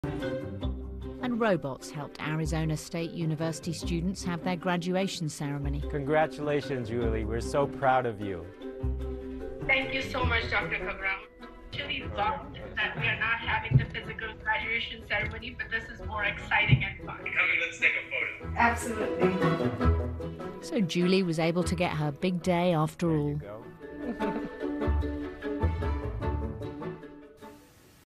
BBCW: Virtual Graduation 2020 Interview with Dr. Sanjeev Khagram
The Thunderbird School of Global Management at Arizona State University celebrated its Spring 2020 graduates in a unique way as the pandemic altered graduation ceremony plans.  Each student was represented by avatar robots to receive their awards and their diplomas from Director General and Dean Dr. Sanjeev Khagram. Learn more about Thunderbird by visiting: thunderbird.asu.edu